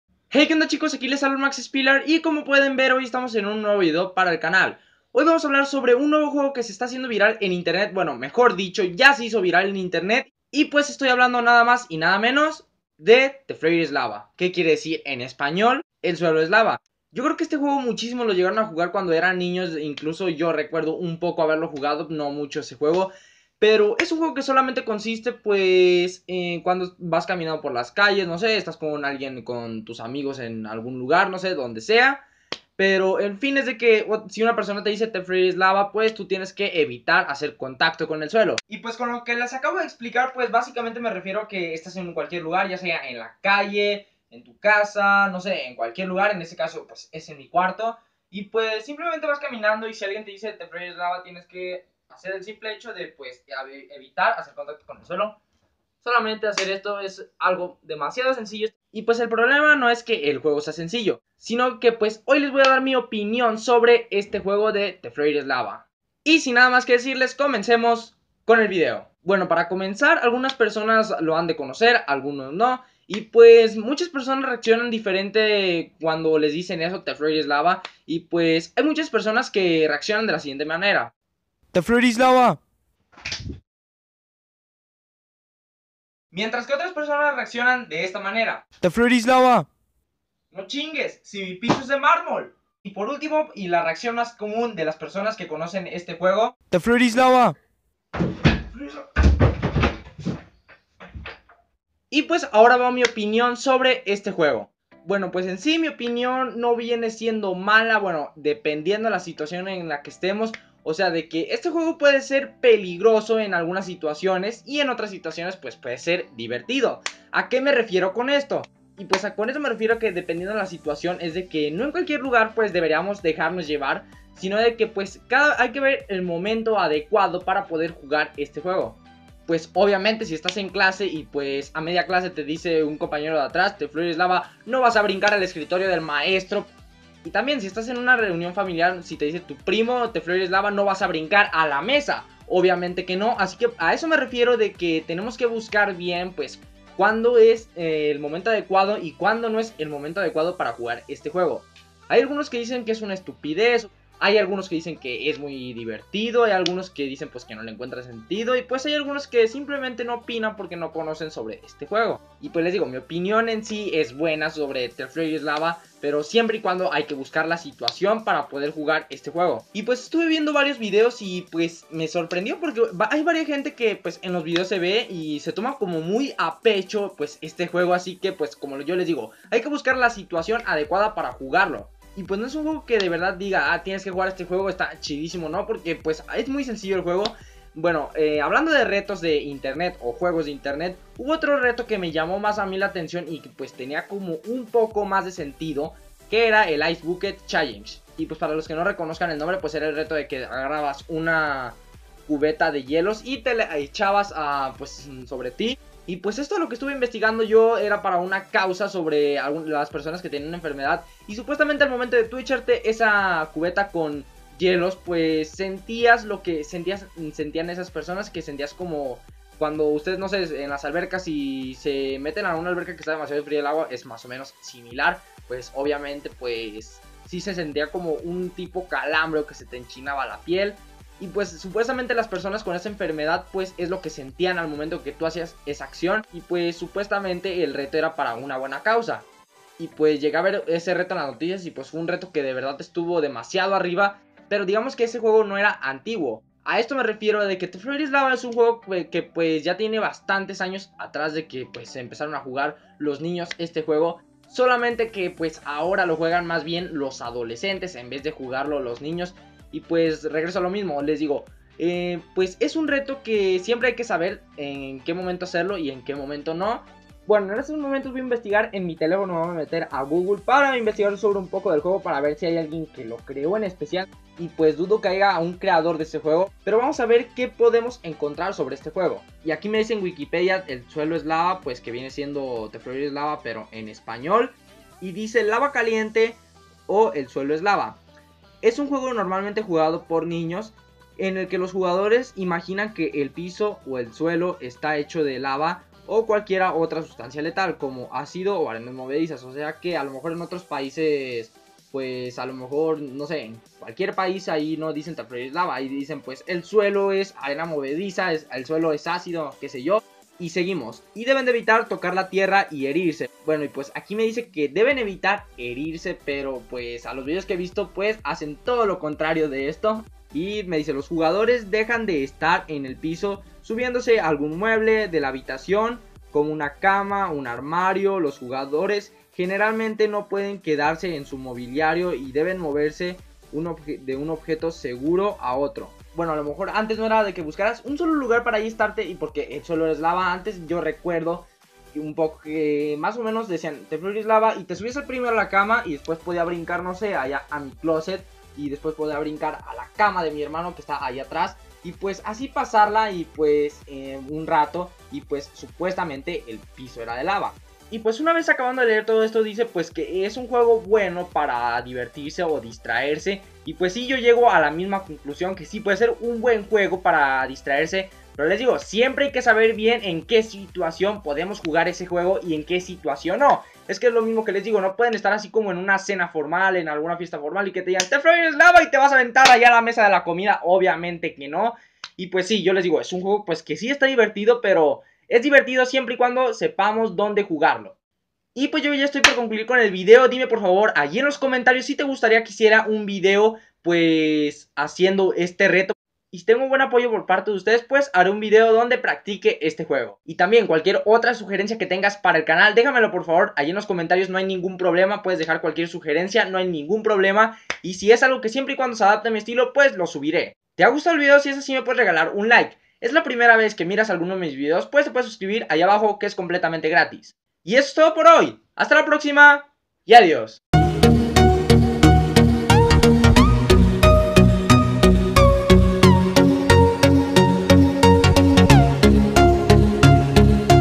The floor is lava (El suelo es lava) mi opinión
En este video hablo sobre el nuevo juego viral en internet llamado the floor is lava que en español es "El suelo es lava"